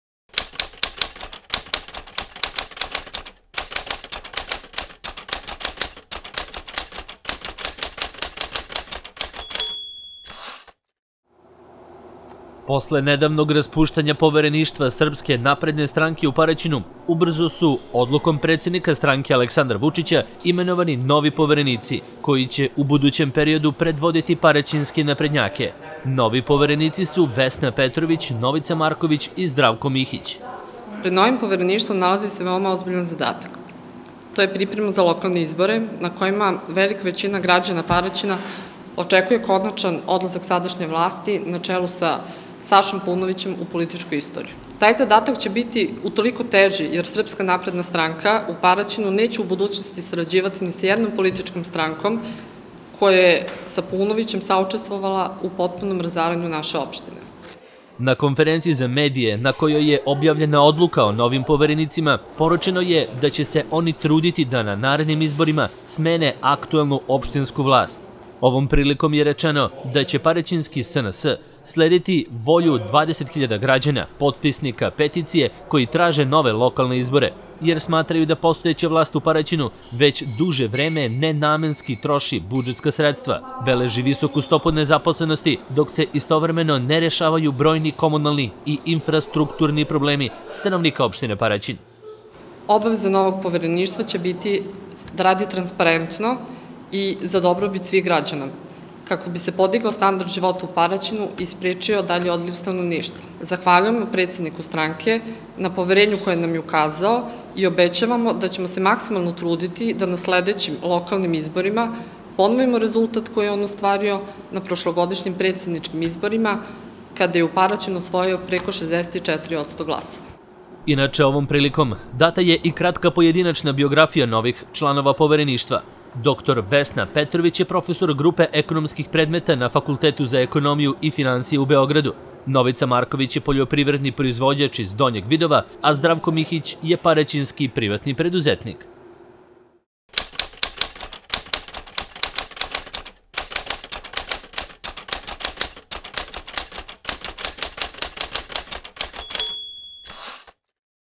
KZM SNS Paraćin
Predsednik SNS Aleksandar Vučić, nedavno je doneo odluku o raspuštanju Povereništva ove stranke u Paraćinu. Na njihovo mesto postavljeni su: Vesna Petrović, Novica Marković i Zdravko Mihić.